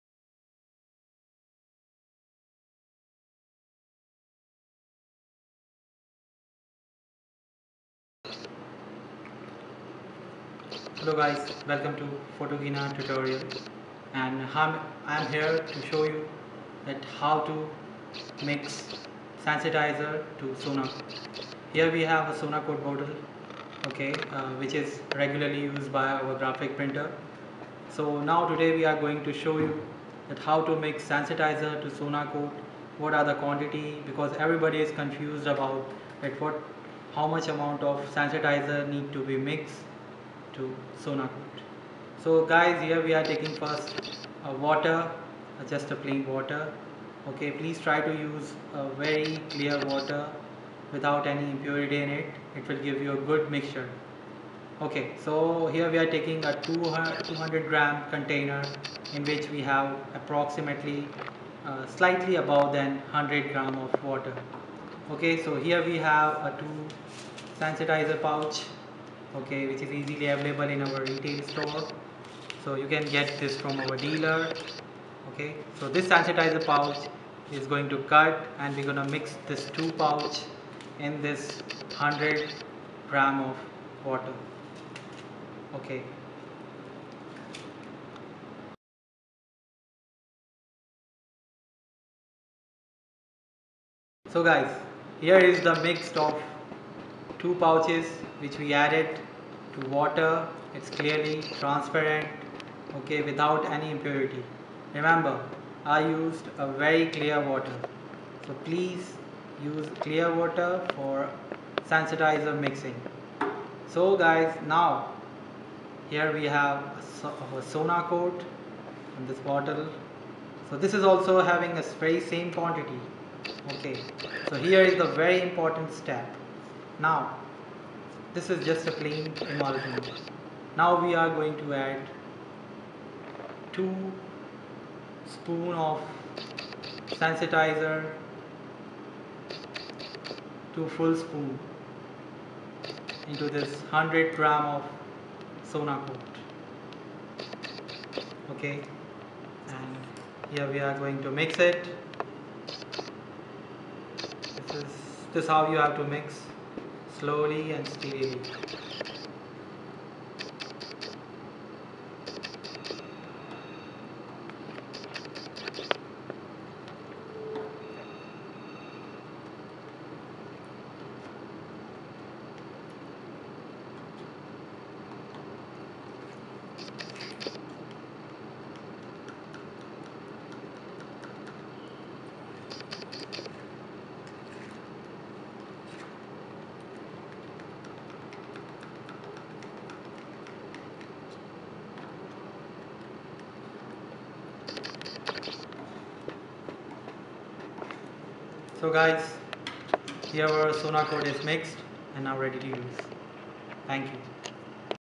How to make Sensitizer liquid and add Sensitizer # Sonakote Screen Printing Hindi
How to make sensitizer liquid and how to make sensitizer.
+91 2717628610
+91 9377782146